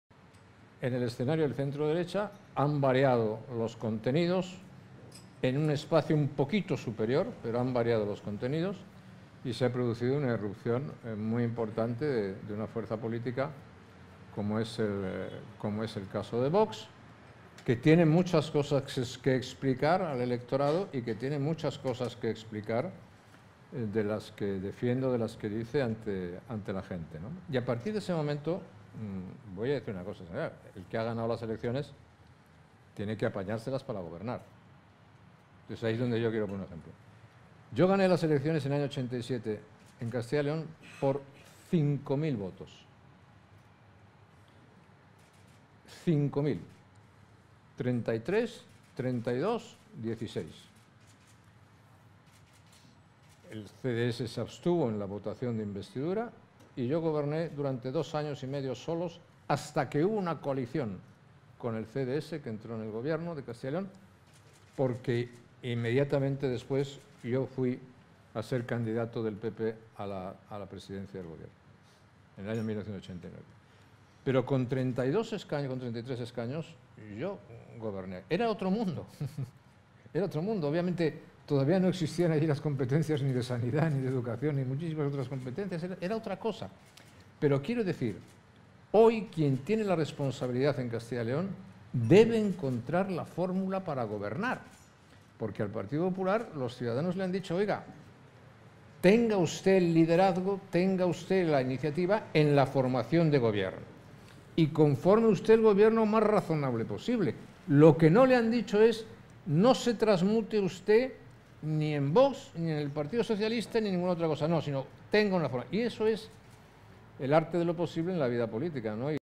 Aznar defiende que el PP forme "el gobierno más razonable"
El expresidente del Gobierno José María Aznar ha defendido este miércoles que es responsabilidad del PP buscar la manera de formar "el gobierno más razonable posible" en Castilla y León, al ser el partido ganador de las elecciones, pero no ha querido opinar sobre con quién debería pactar.